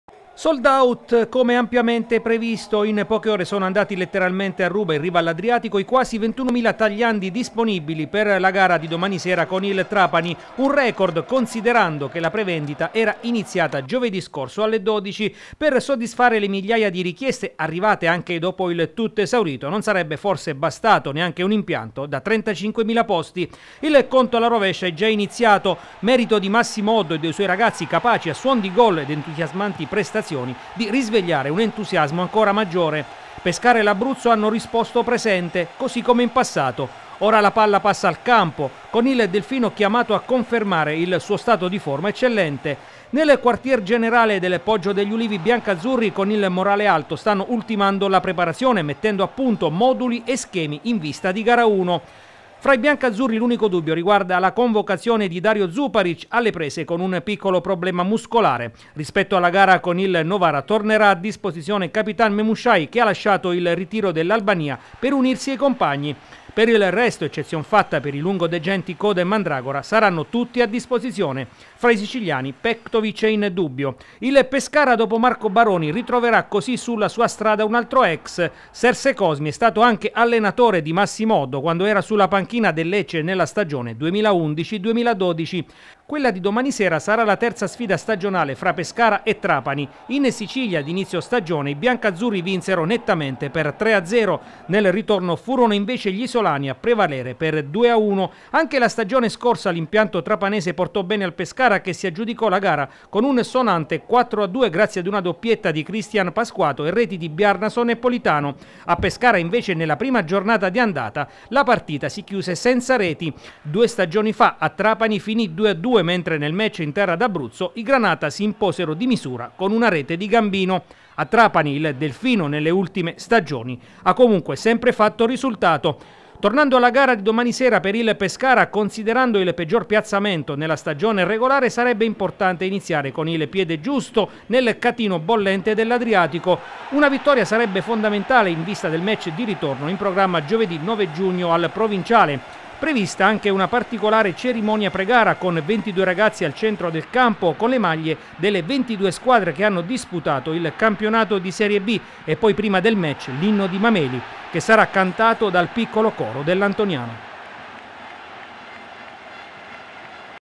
Il Pescara aspetta con fiducia il Trapani
Pescara aspetta con ansia la gara di domani sera con il Trapani. La squadra sta ultimando la preparazione al Poggio degli Ulivi mentre i tifosi stanno organizzando grandi coreografie nel corso del match. Fra i biancazzurri in dubbio Zuparic. Nel Trapani  Petkovic non è al meglio.